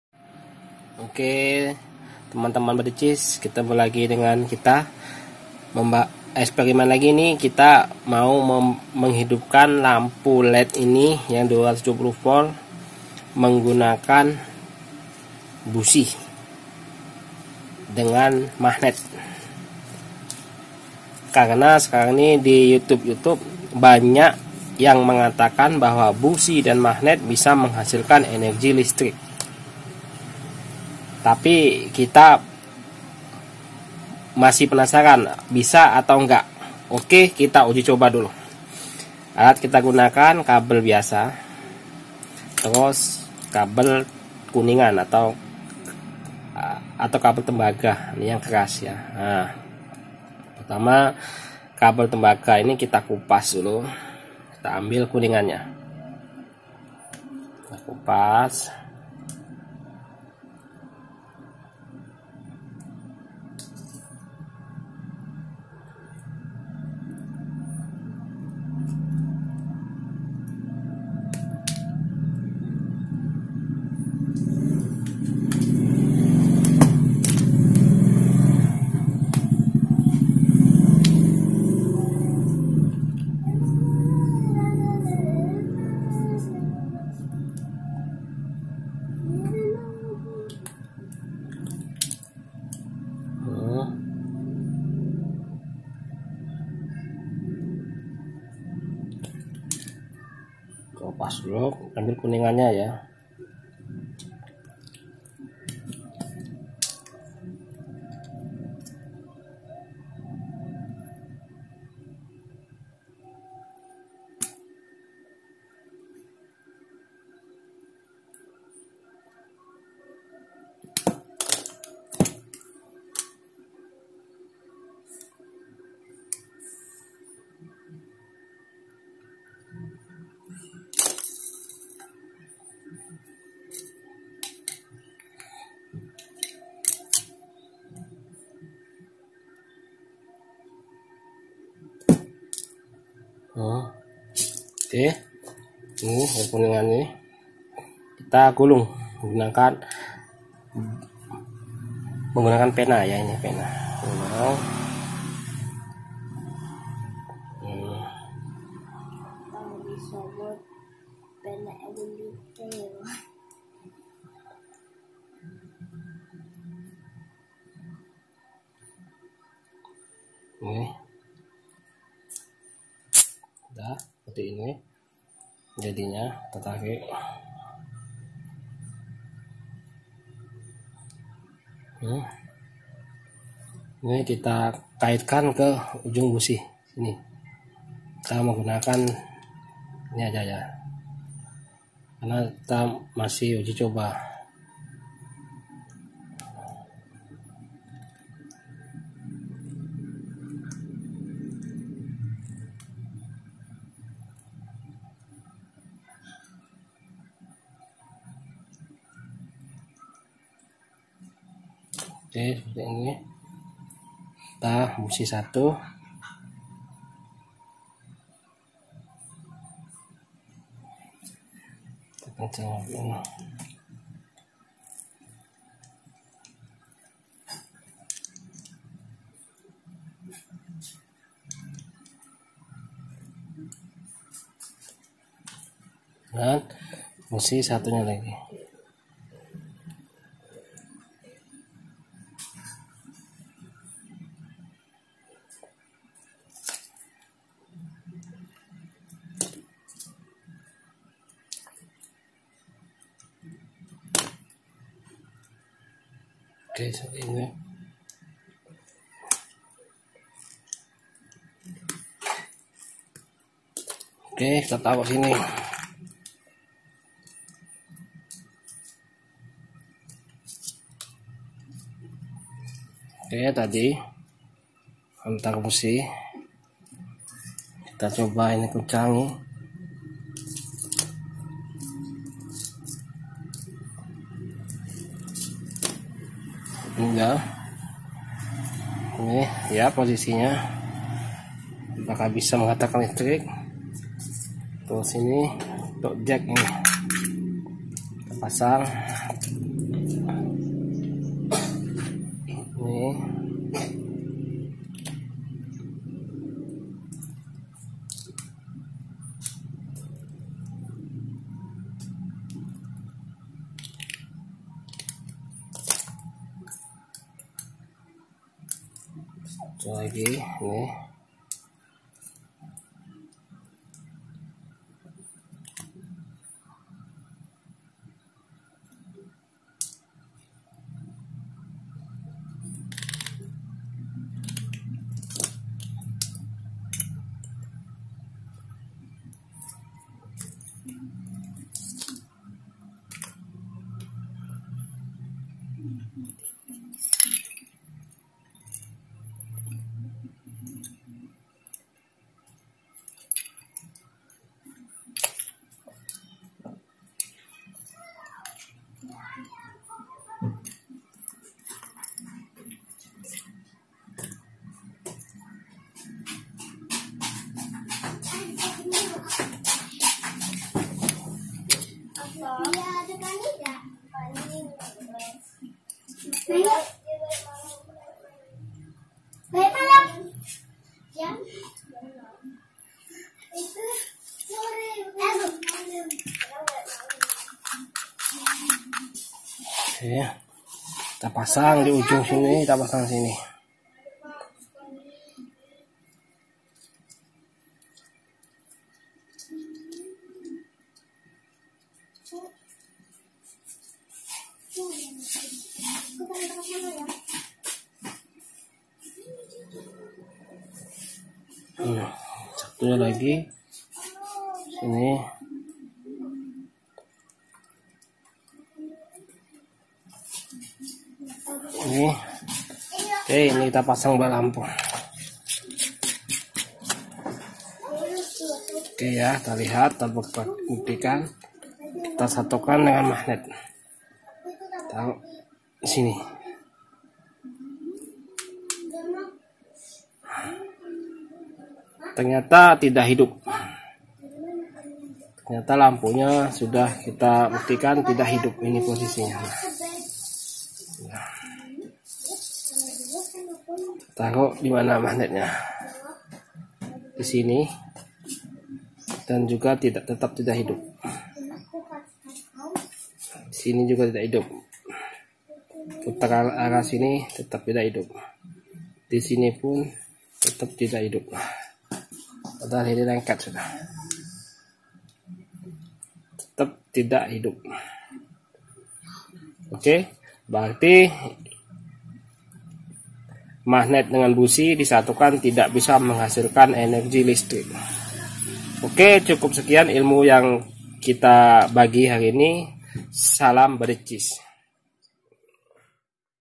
Wow!! FREE ENERGY DARI BUSI BEKAS HO4X atau FAKTA?
Free energy dari busi bekas apakah benar atau tidak benar? Maka dari itu saya mencoba untuk membuktikan apakah free energy dari busi ini bisa menghasilkan listrik. Semua jawaban dari fakta bahwa video free energy busi ini benar atau tidak ada dalam video. So, silakan tonton video free energy ini sebelum komentar dan jangan di skip biar tidak salah faham.

Elektro Channel, Rangkaian Elektronika, Rangkaian Lampu